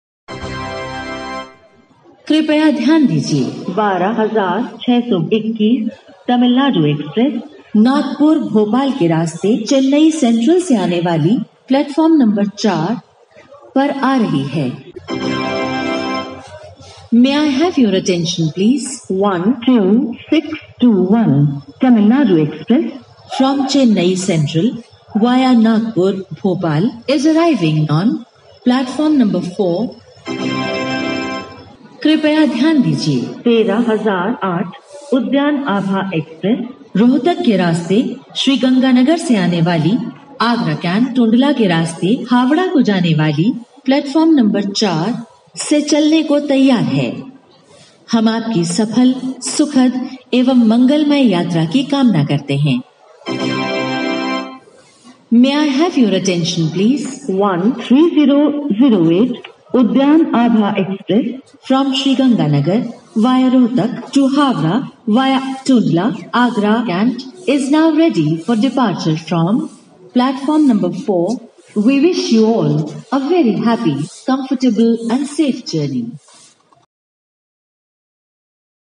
Live !! Popular Indian Railway Latest & Clear Train Announcement at New Delhi 2020 : Part 5
Train Announcement 2020

Who is Indian Railway Announcer?
यह किसकी आवाज़ में Announcement हो रही है?-

Tamilnadu Express
Udyan Abha Toofan Express !!

❤️❤️❤️ Rail Guruji ❤️❤️❤️